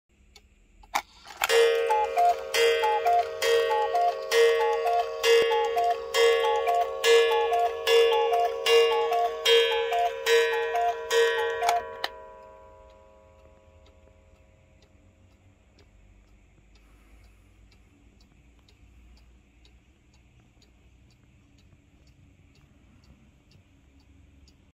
Bears & Honey 8 Day Mechanical Chalet Cuckoo Clock 40cm By Hönes
Bears & Honey 8 Day Mechanical Chalet Cuckoo Clock 40cm By Hönes (Hönes 8709 KO)

Measuring 40cm in height*, this impressive hand-carved clock features two mischevious bears out the front, alongside a pot of honey. Surrounding them are a number of carved fir trees that stand tall - such detailed forest foliage frames the clock perfectly. The base of the clock is another feature that has been heavily hand carved, with the overall timepiece finished in an antique walnut colour.

The clock also features a timber facade and a chalet style roof, with individual hand laid wooden shingles, along with a wooden clock dial in the centre of the piece, and a timber pendulum that hangs below the clock itself, alongside the weights. Rounding out this fascinating timepiece is a bell tower that stands tall above the chalet roof. This particular clock runs on an 8 day mechanism, and therefore needs to be wound once a week simply by pulling the weights. The in-built cuckoo of this clock calls the time each hour and once at half past, and has a manual silencing switch should that be preferred. This clock does not play any music.